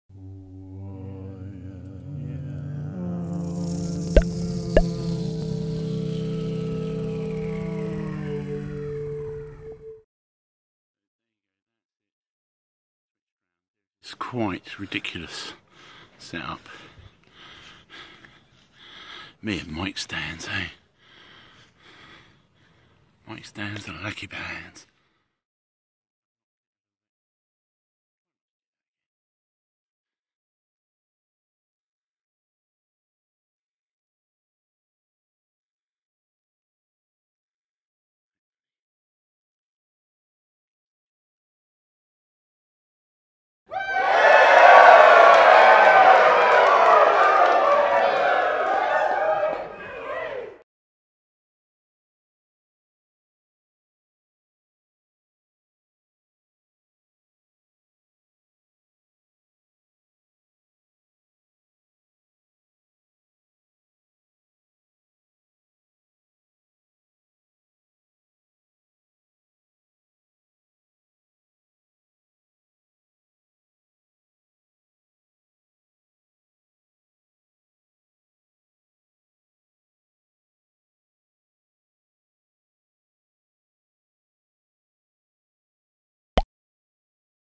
How to Capture a Sunset! (Camera + Info)
In the summer I had several goes at capturing sunsets with timelapse on my iPad II. This was one of the more crazy setups!

captainflyingcarpet flyinghinges.co.uk

nielsencs